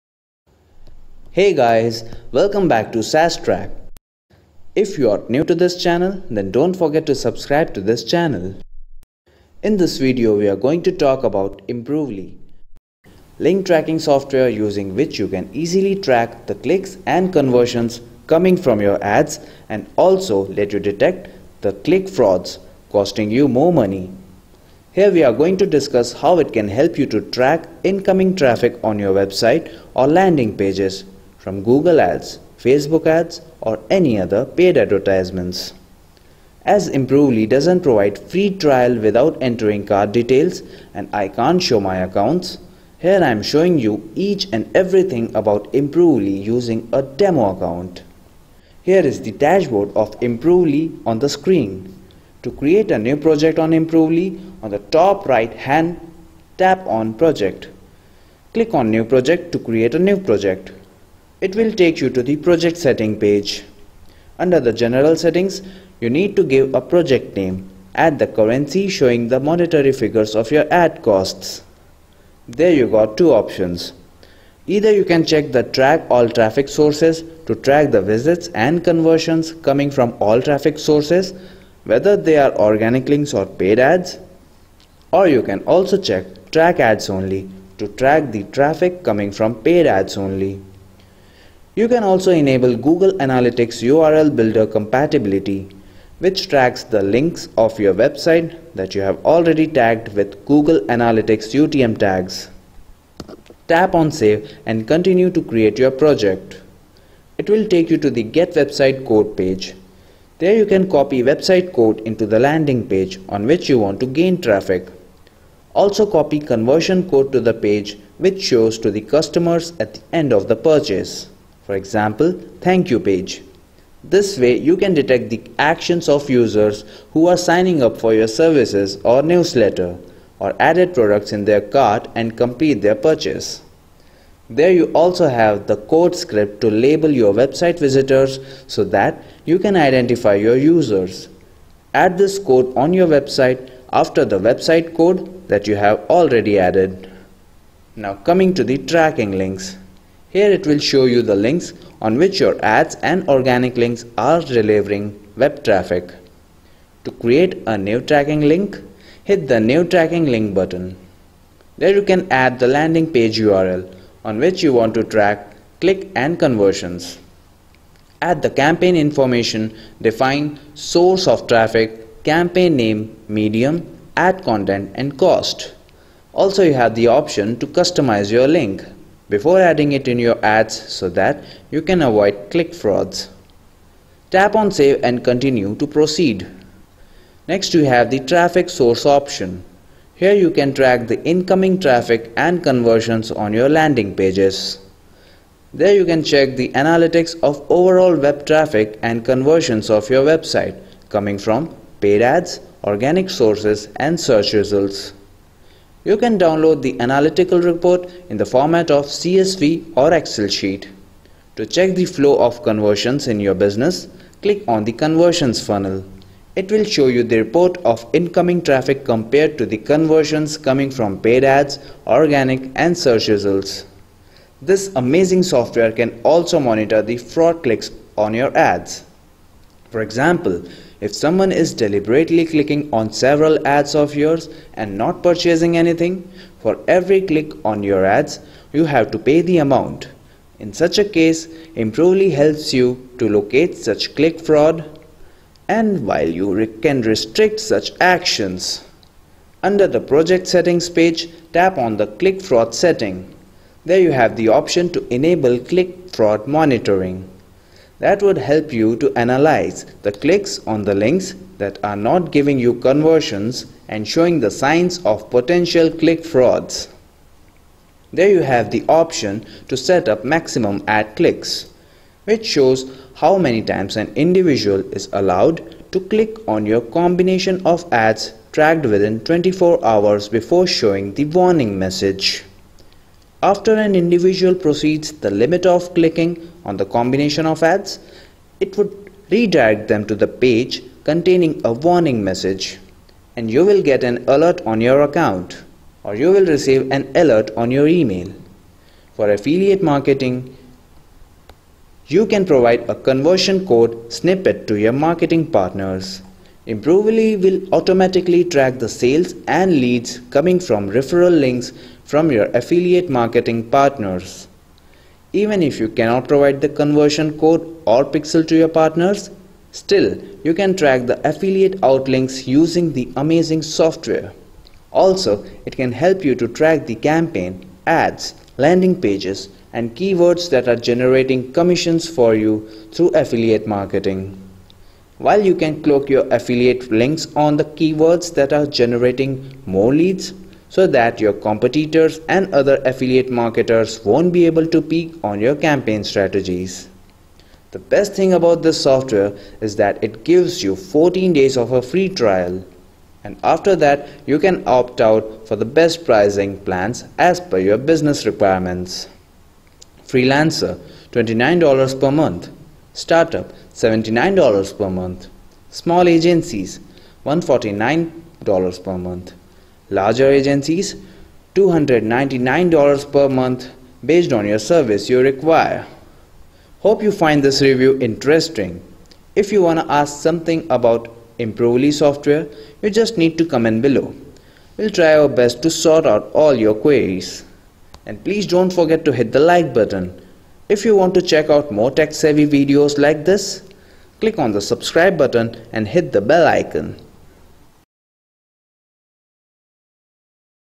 Improvely Review: Best Conversion Tracking & Fraud Click Monitoring Software?[Dis. Coupon Inside]
In this video, we have reviewed Improvely. Improvely is an amazing software for monitoring conversion results and detecting fraud clicks.

We will check everything about Improvely. Watch this video till the end to explore more!